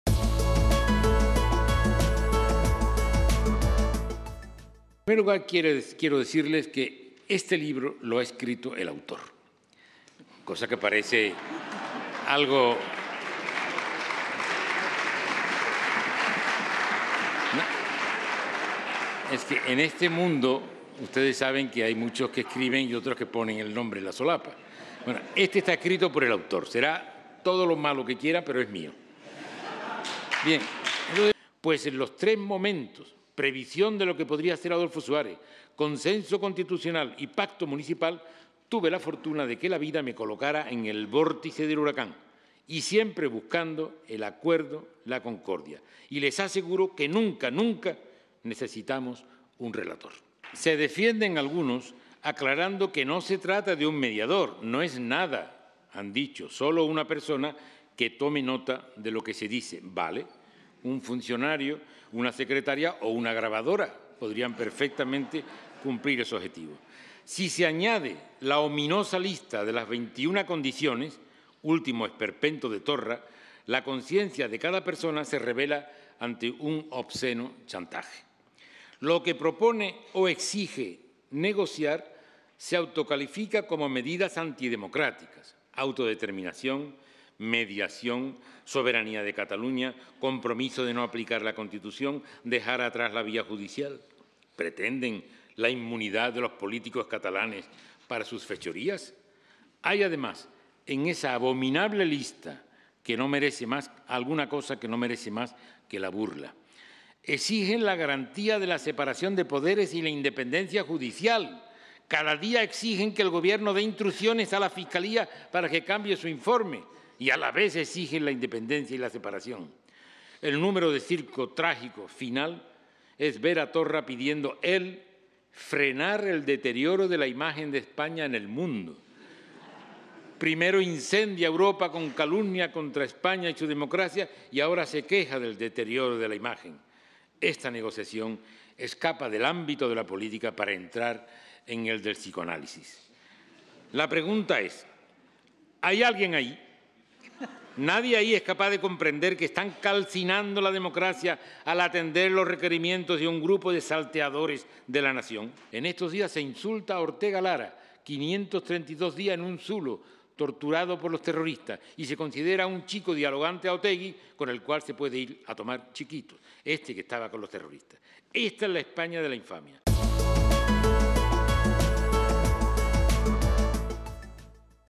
Alfonso Guerra destroza a Pedro Sánchez en la presentación de su libro
Alfonso Guerra ha cargado contra el PSOE y Pedro Sánchez durante la presentación de su libro 'La España en la que creo'.